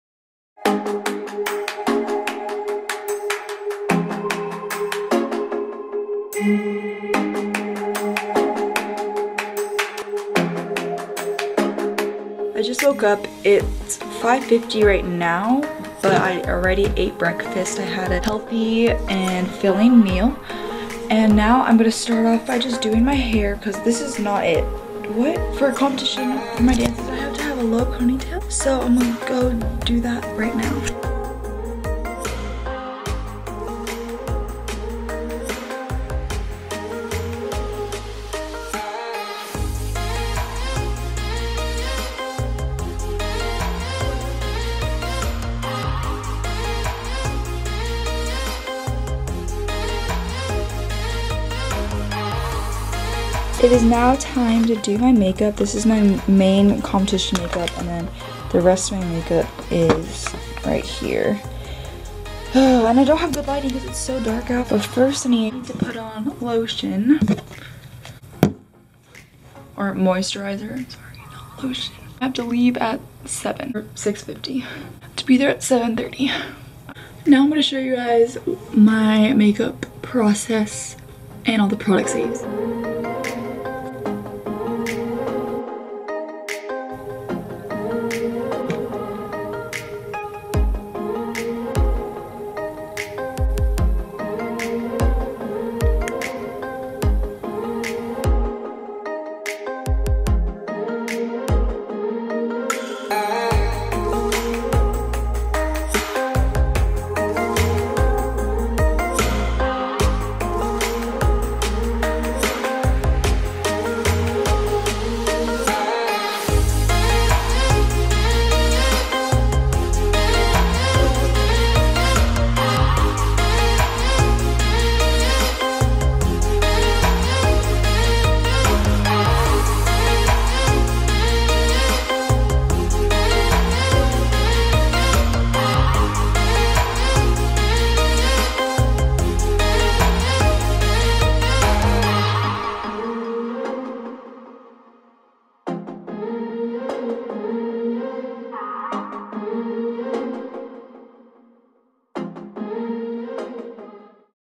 GET READY FOR A DANCE COMP WITH ME
Hey logicalistics! Sorry for the really short vid. For this weeks video, I decided to show you all the makeup and hair products I use the morning of a dance competition. Hope you enjoy :)
While you're here, click on these links to stalk Lynsey Logic...
Leave me some video ideas in the comments ;)
Bye!!